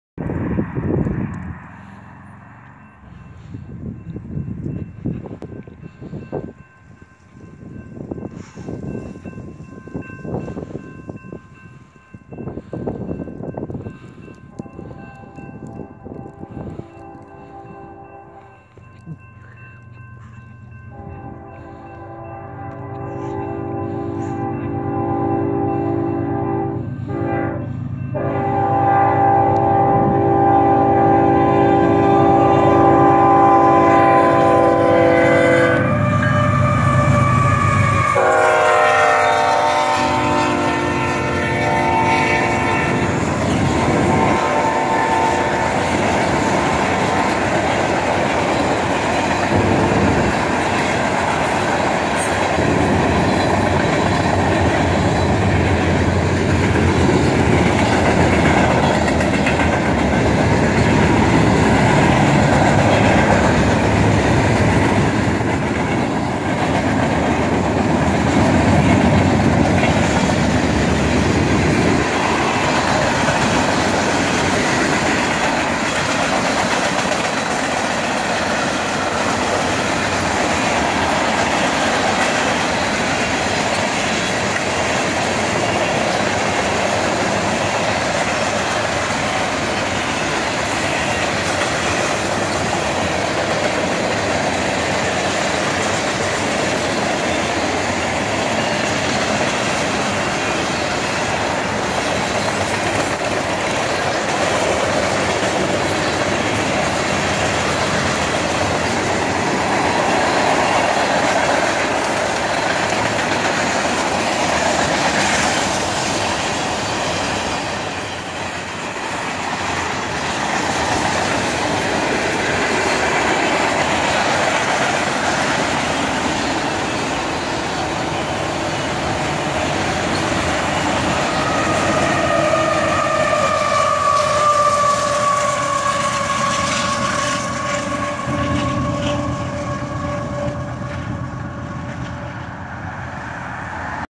WB Union Pacific 8028 Leads Manifest Train in El Paso, TX
List of the Engines:

UP 8028 GE (C45ACCTE) Leading
UP 5806 GE (C44ACCTE)
UP 4249 EMD (SD70M)
UP 5510 GE (C45ACCTE) DPU
UP 7163 GE (C44AC) DPU